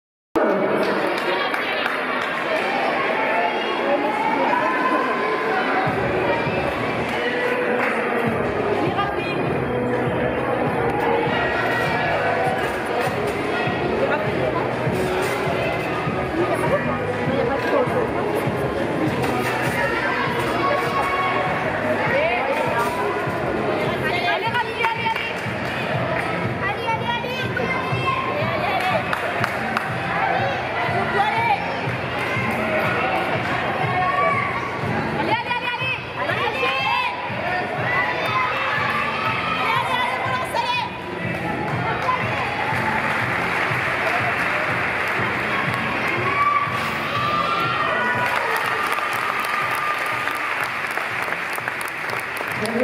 Raphaelle Letort - finale barres junior - Championnat de France elite 2023
12,250 points / 4e